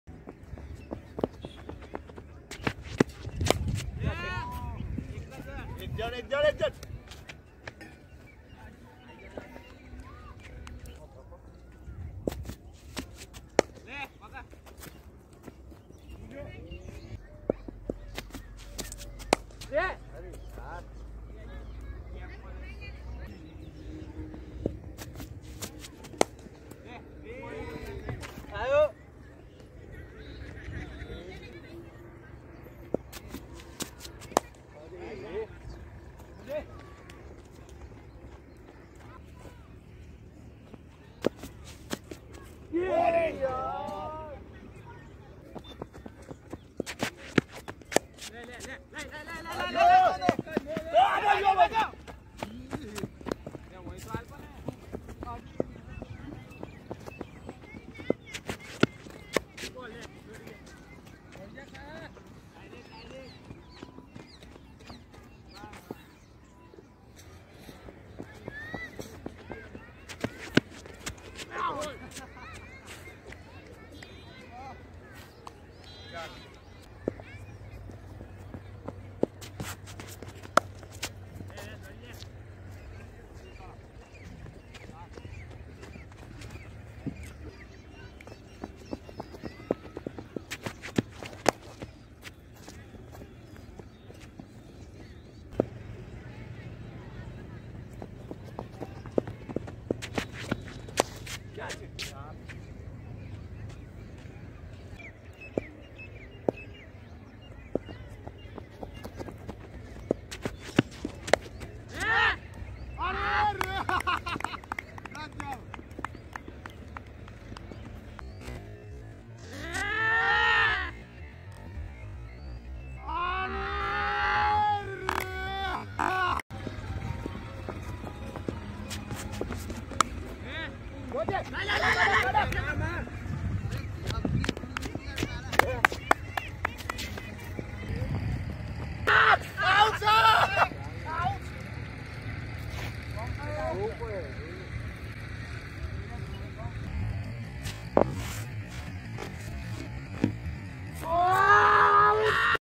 highlights of morning leather ball cricket practice session 05.10.2020
cricket lovers
practice session
cricket practice
morning practice